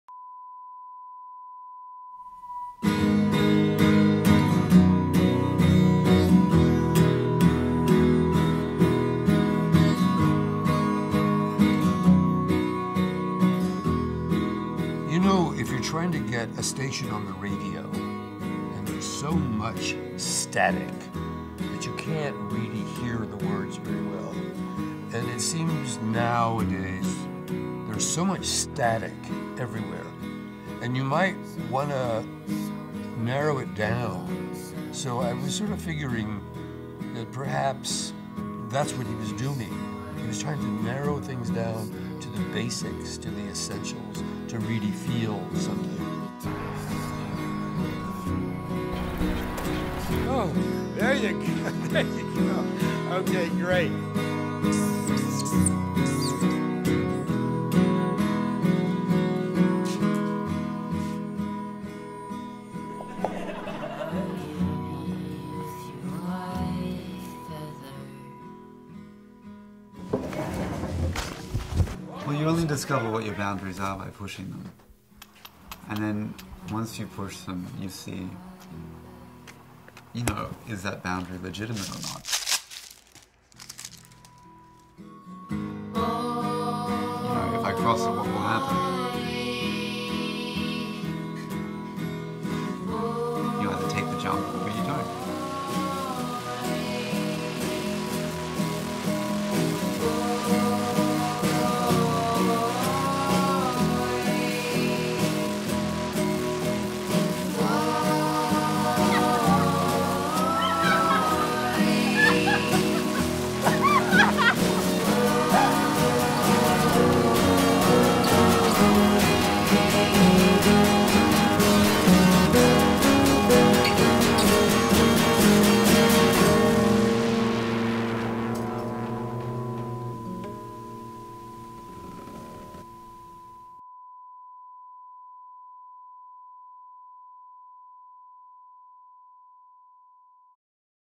Simple Being
Three weeks for three impediments: deafness, mutism and blindness. Three weeks not to escape, but to find reality.